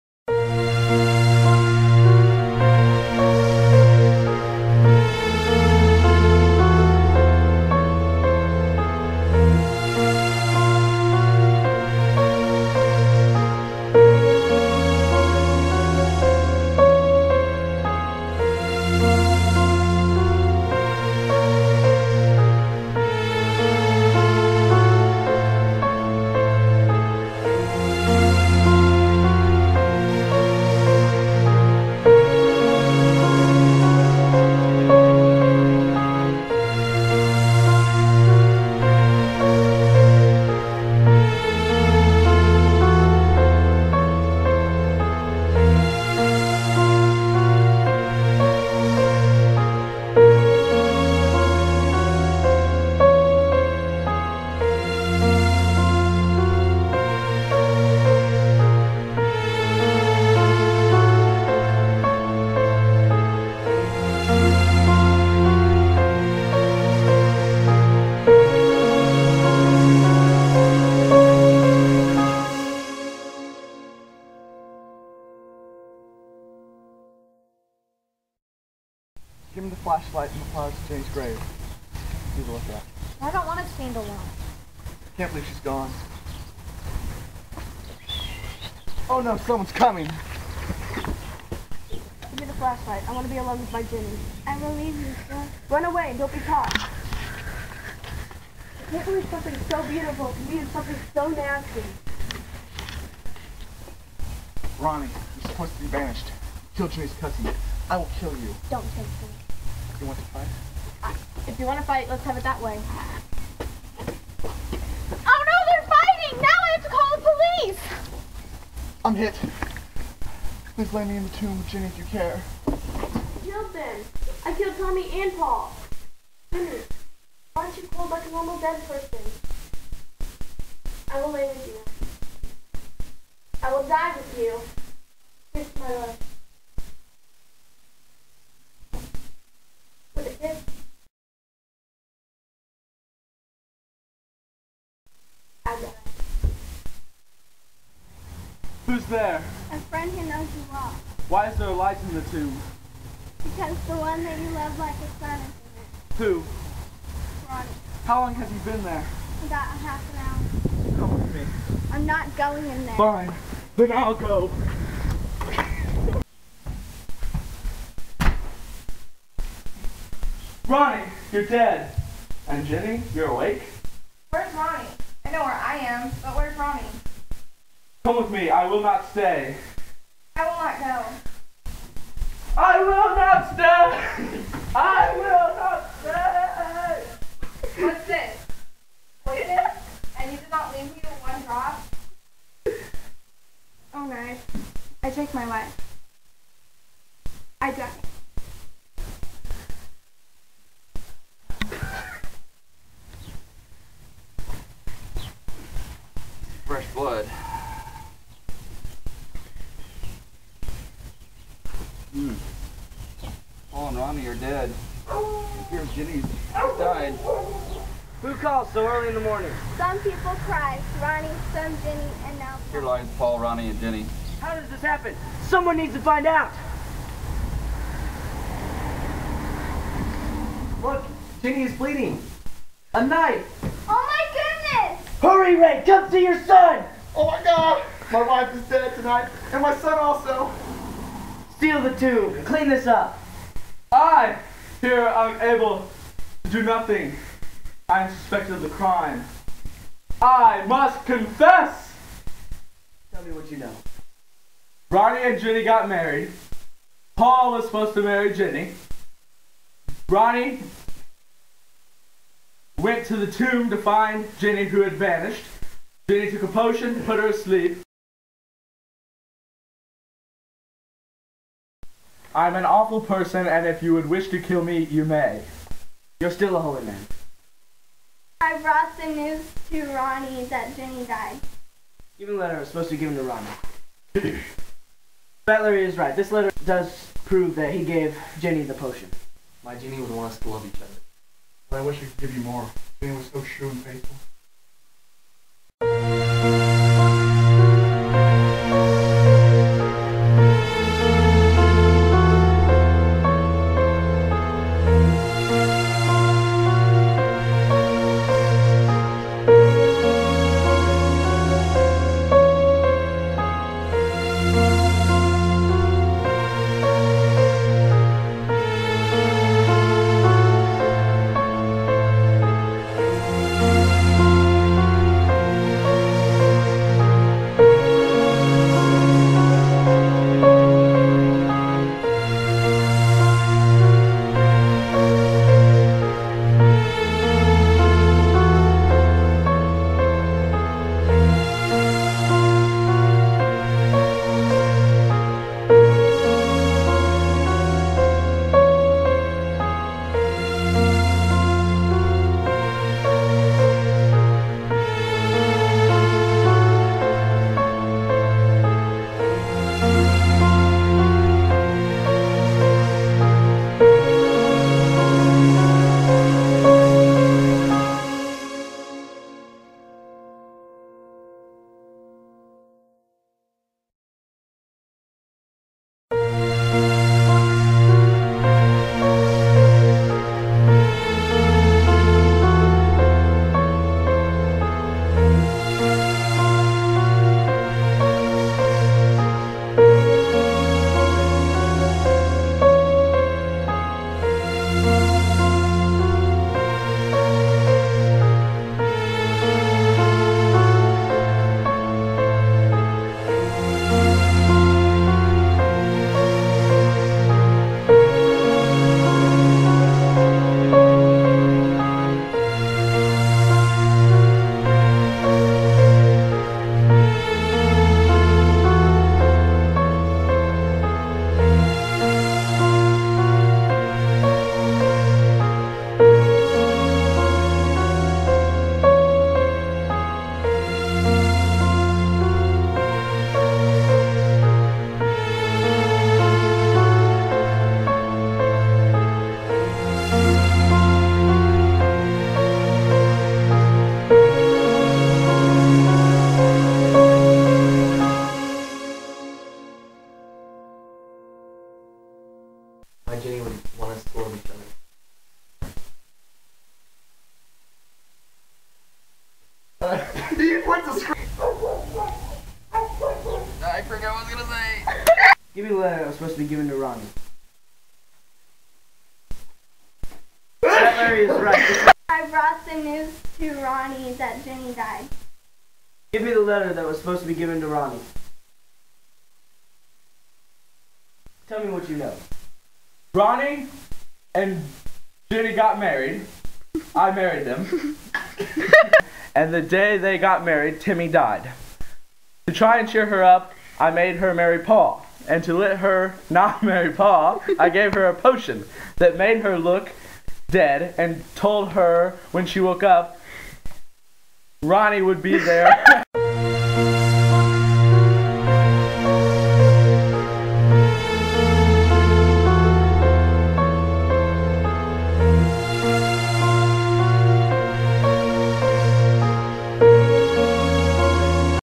Romeo and Juliet Parody
This is a parody I made for school, with some of my friends